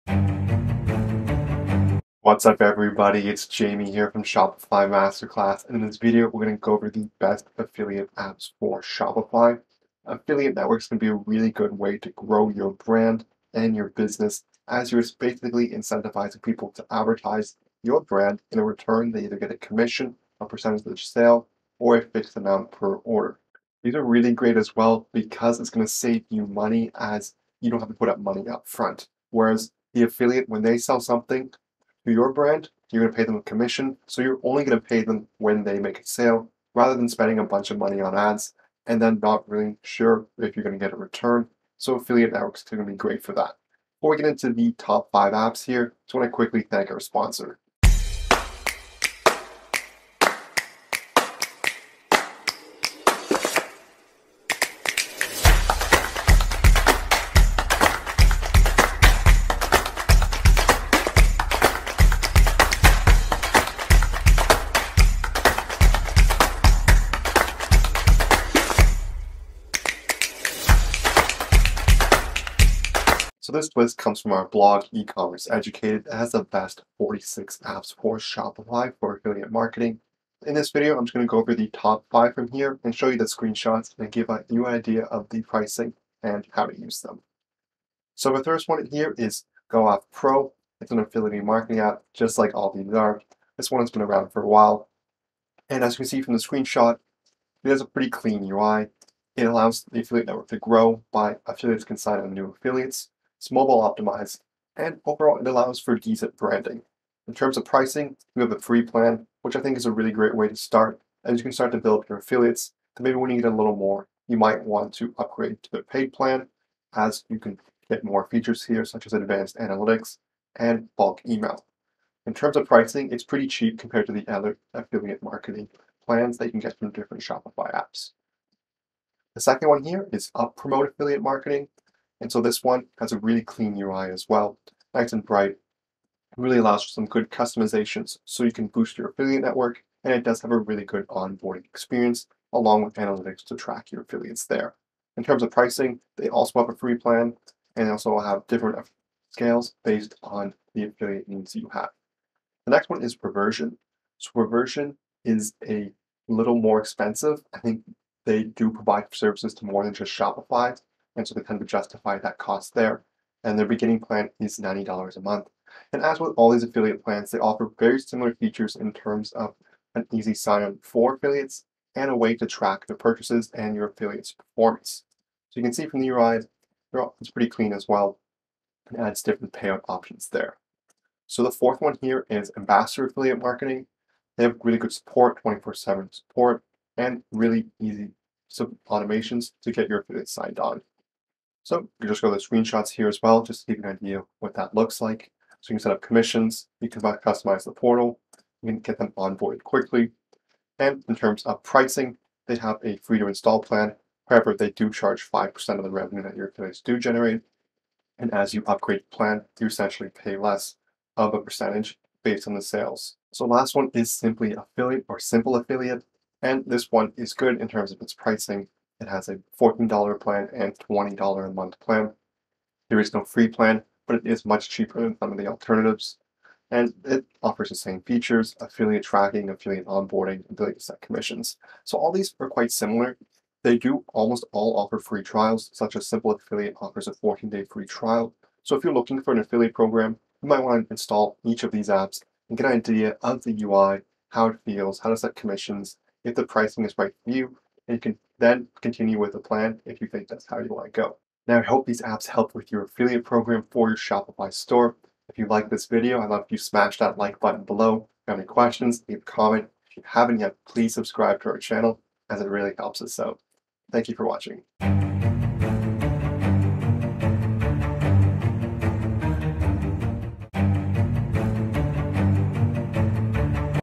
Best Affiliate Marketing Apps for Shopify
We're going to talk about the best affiliate marketing apps for Shopify. These apps will make it easy for you to track your affiliate sales, create custom affiliate links, and more!

Affiliate networks are an easy method to boost your brand's presence online without having to pay a lot of money upfront for online ads. With affiliate programs, you can share the revenue from any sales you make with your affiliates. Creating a win-win for both of you.